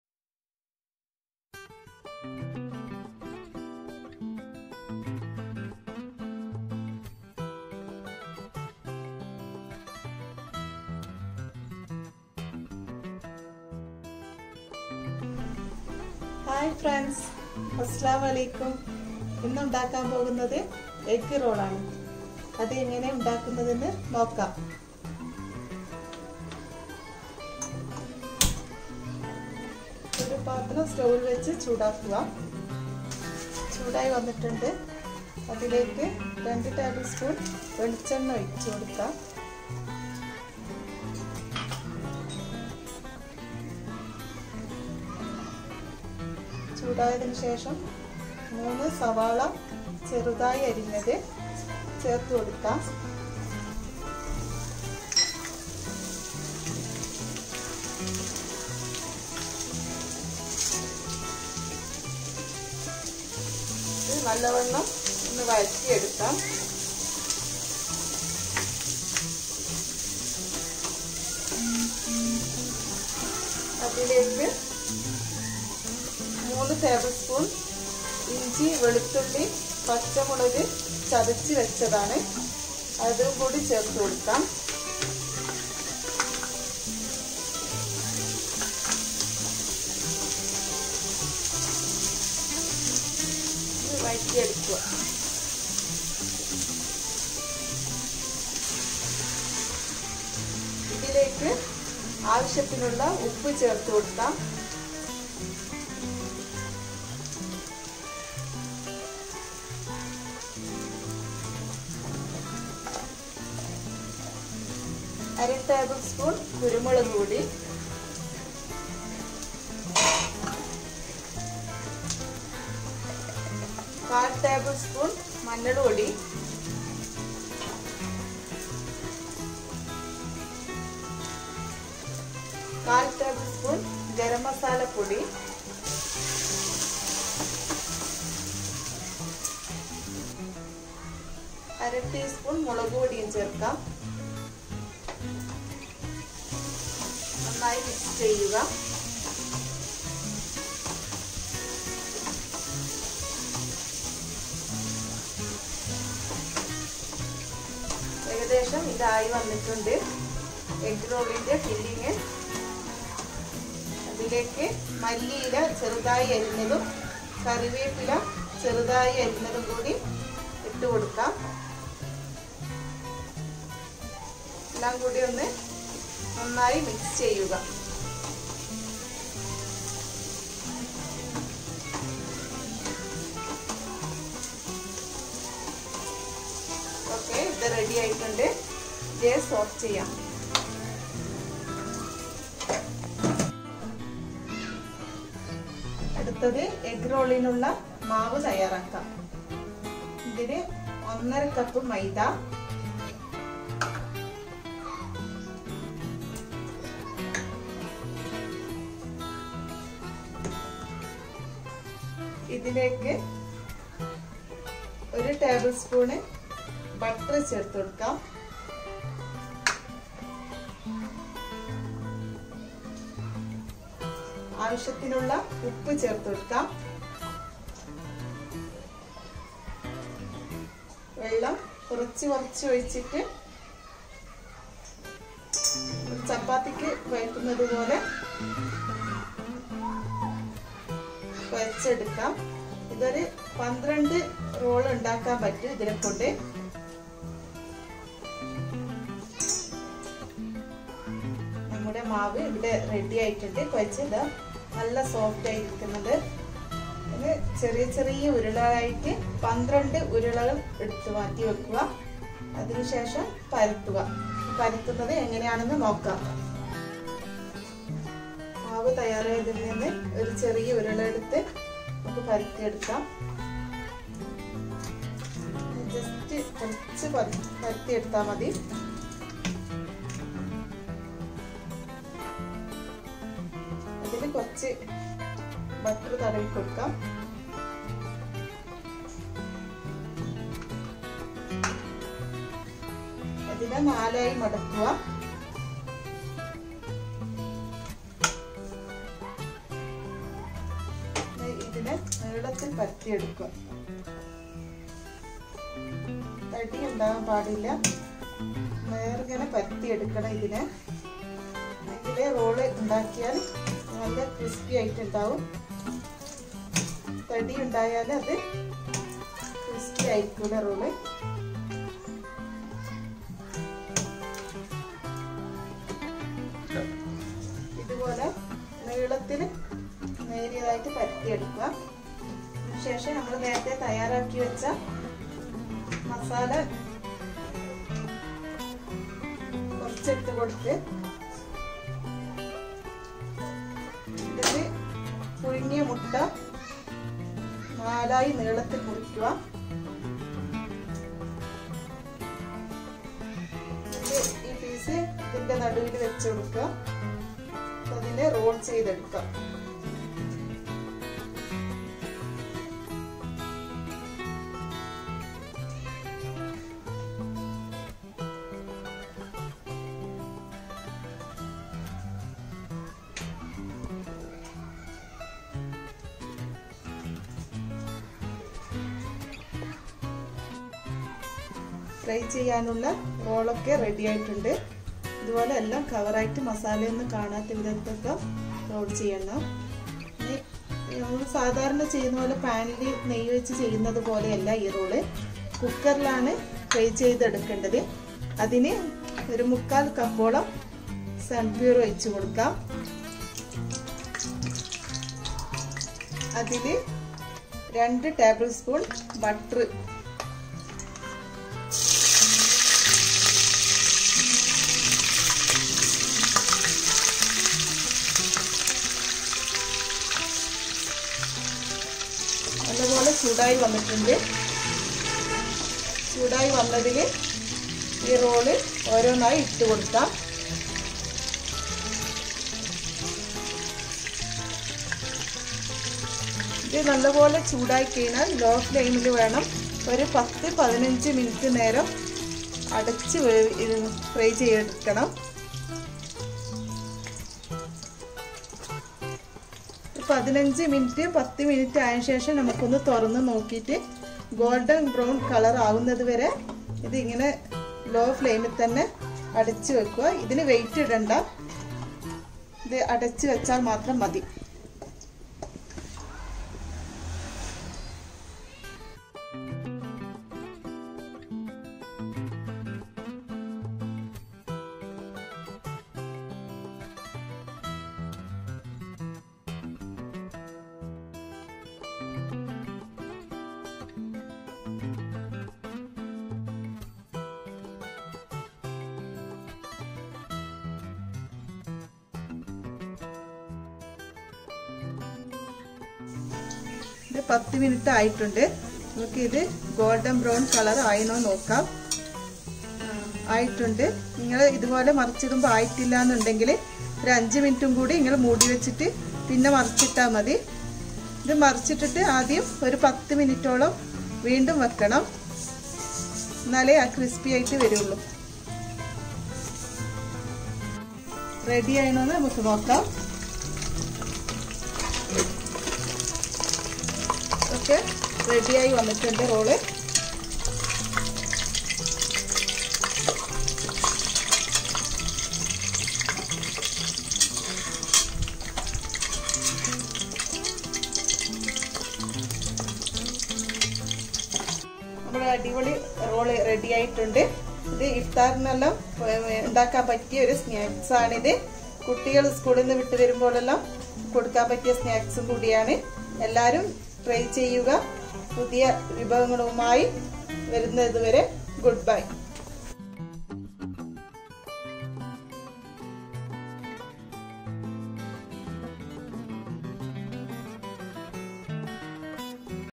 Egg Roll Fry | Mubeena Nazar | Quick bite | Evening Snack
This video shows how to prepare an Egg Roll Fry. Egg Roll Fry is a Quick bite. It has that crispiness out and hot tasty egg masala inside! Food Studio conveys best wishes to you on preparing the dish!

With love,
Food Studio